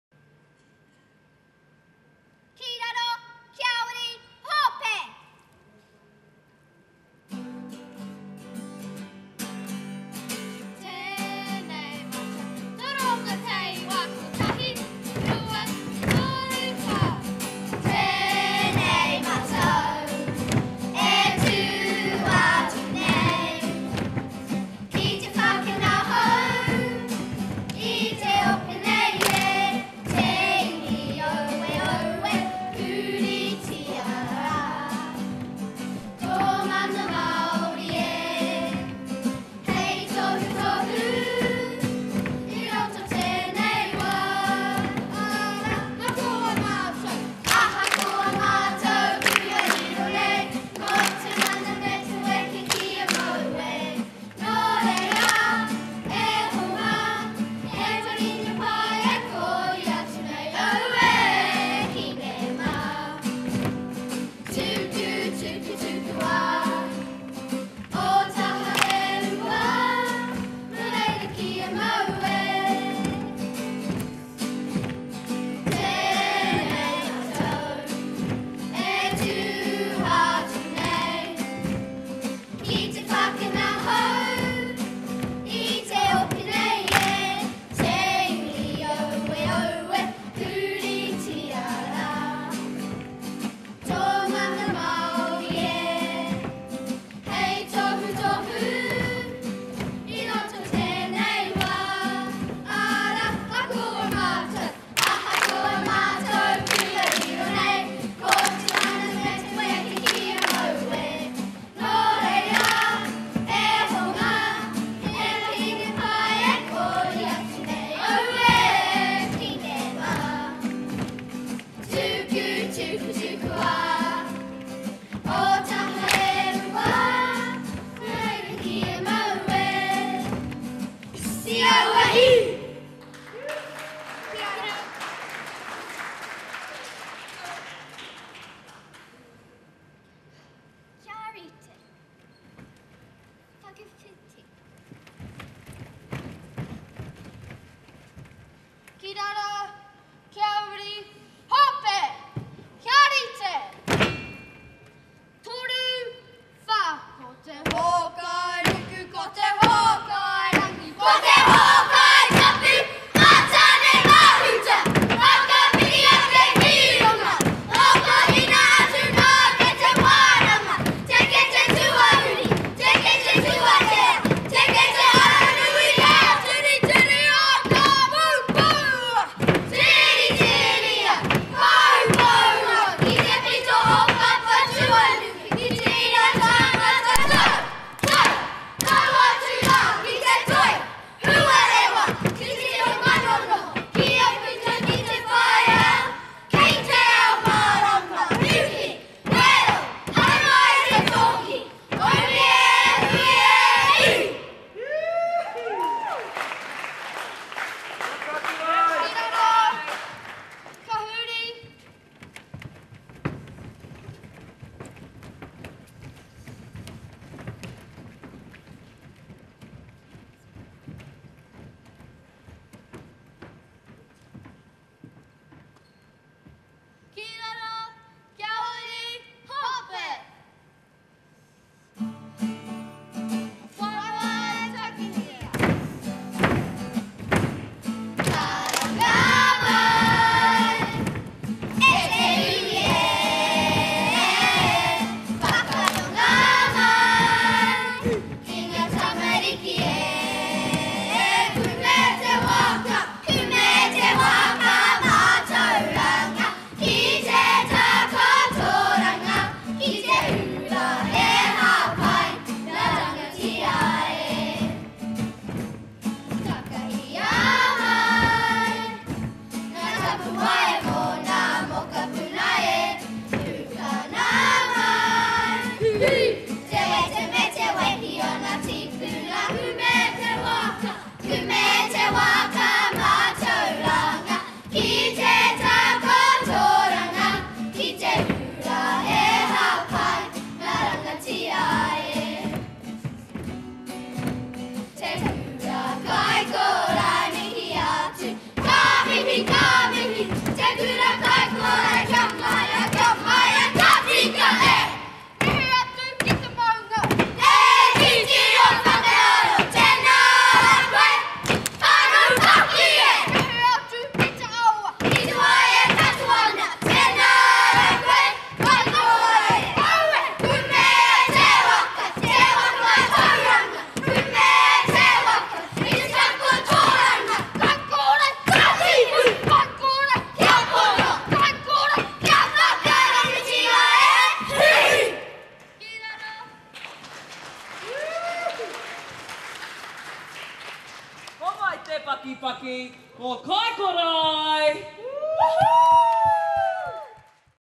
Kaikorai School Kaikorai Tuakana: Otago Polyfest 2021
Otago Polyfest 2021 is different. Run under Covid 19 Level 2 conditions with no live audience and livestream only. The tamariki adapted as tamariki do and so this vibrant celebration of Māori and Pasifika cultural performances including kapa haka and waiata performed by groups from Otago schools,  was held at the Edgar centre in Ōtepoti Dunedin. Since its beginning in 1993 the Otago Polyfest has become a significant annual cultural event for Otago preschools, schools, high schools and whanau, encouraging participation and positive cross-cultural relationships. Everyone is welcome – nau mai, haere mai.
Polyfest 2021